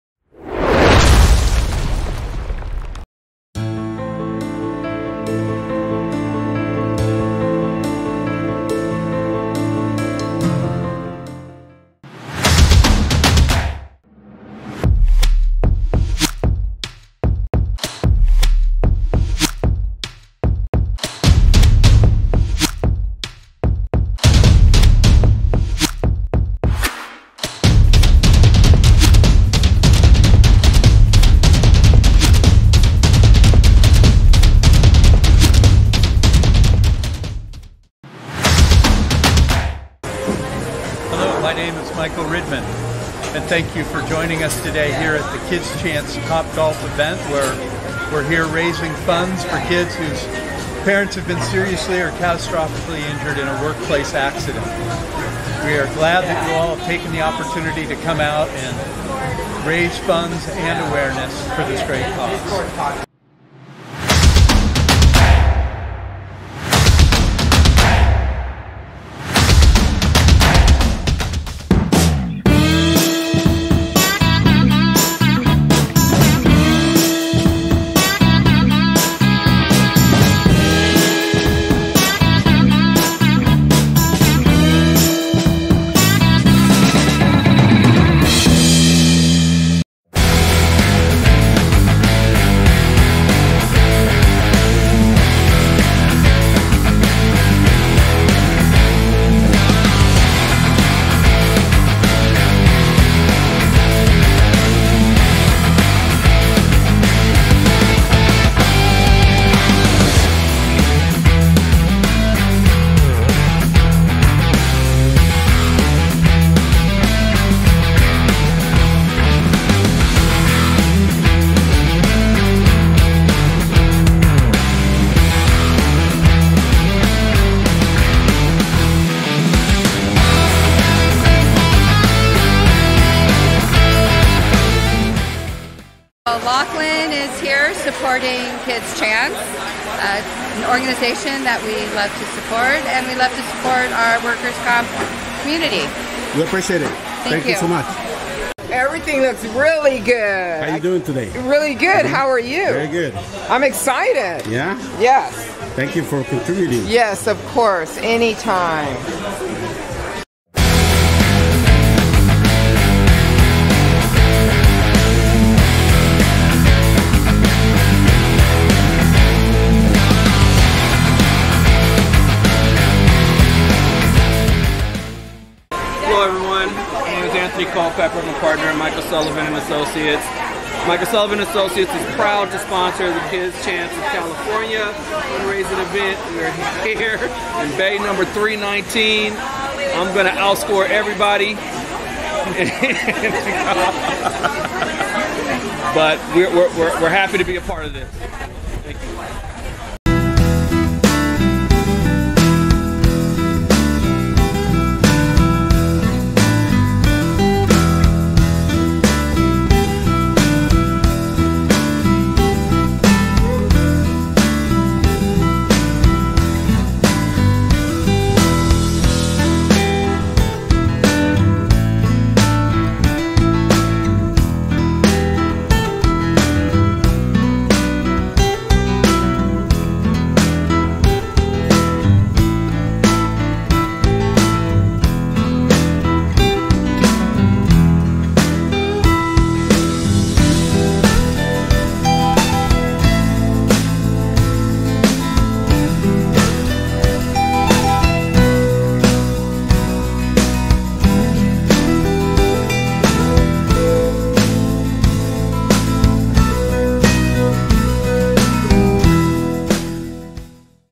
KIDS CHANCE CALIFORNIA Top Golf Fundraiser 2023
This video has the images of the KIDS CHANCE Golf Top event that took place on August 9, 2023. It was an honor to be there to capture the smiles and fun of all attendants. It sure was a full house. We thank everyone for making this event a memorable evening for an amazing cause.